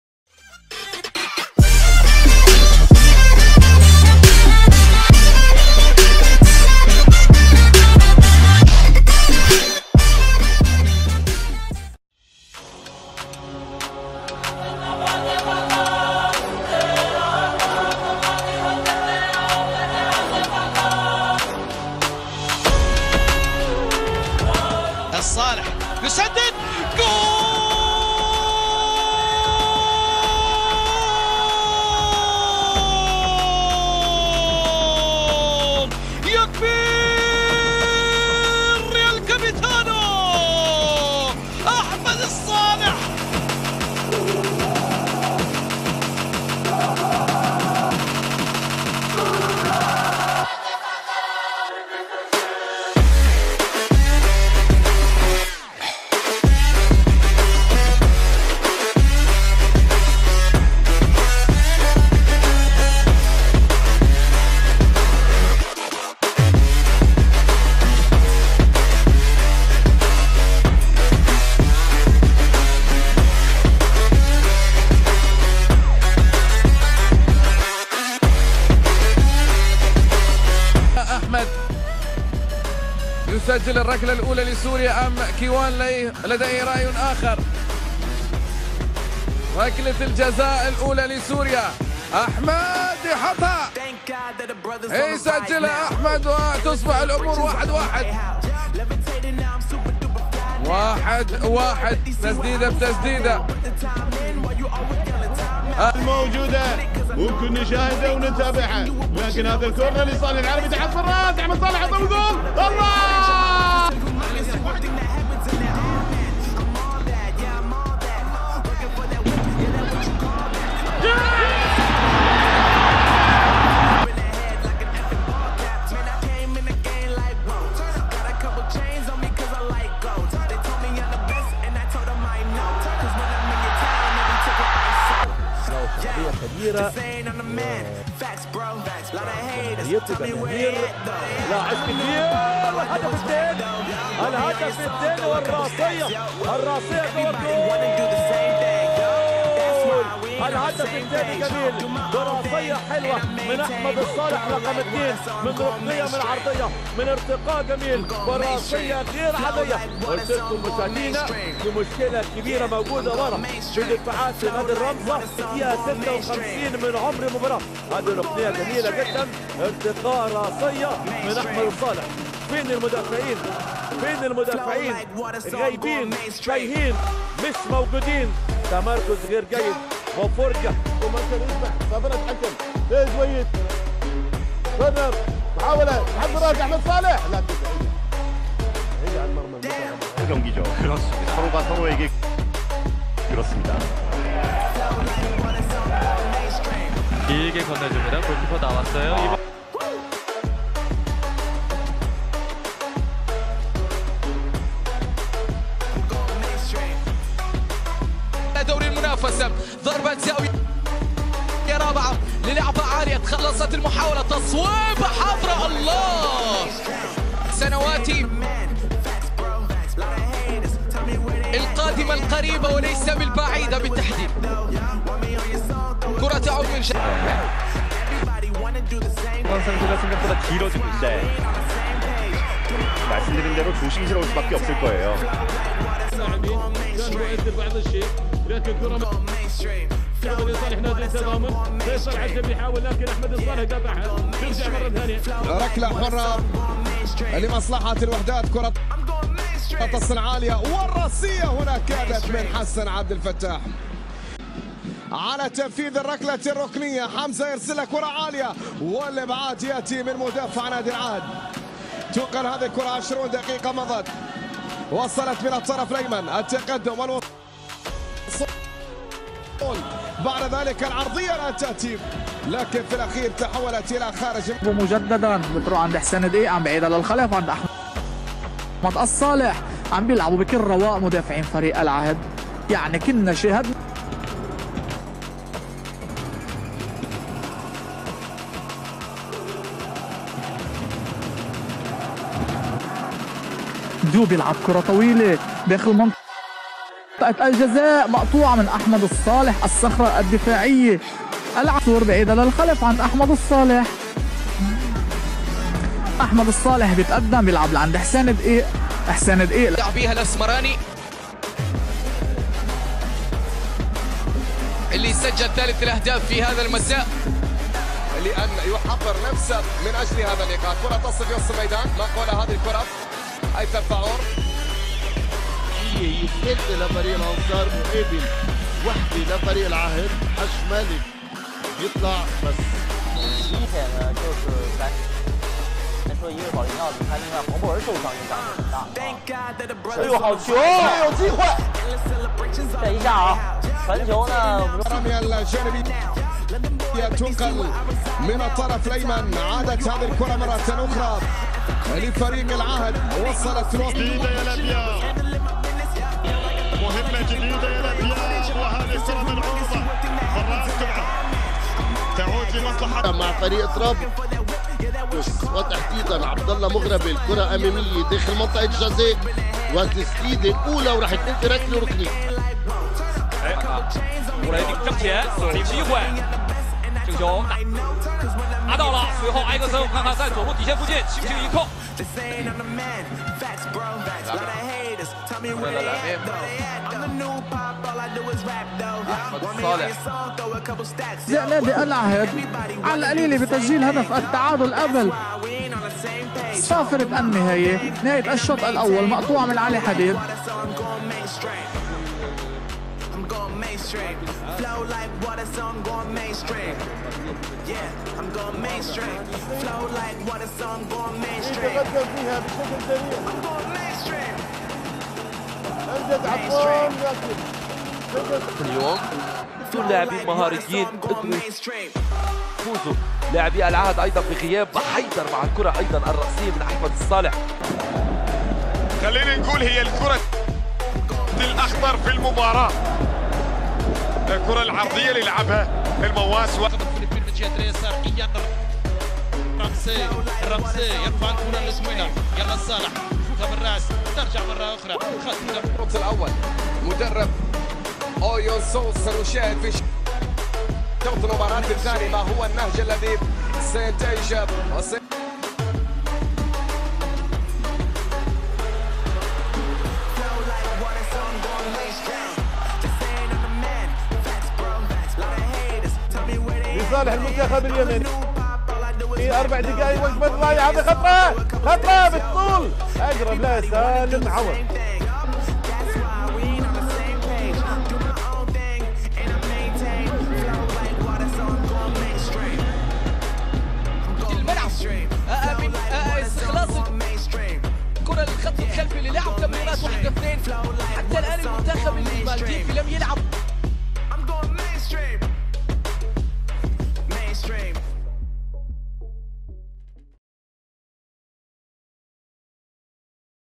AHMAD AL SALEH | The Art of Defending & Goals
Full name: Ahmad Al Saleh
Date of birth: 20 May 1989 (age 31)
Place of birth: Amuda, Syria
Height: 1.83 m (6 ft 0 in)
Playing position(s): Centre Back

Current Club: Al-Arabi, Kuwait [2019 to Present]
Shirt Number: 3

National Caps: 54
National Goals: 2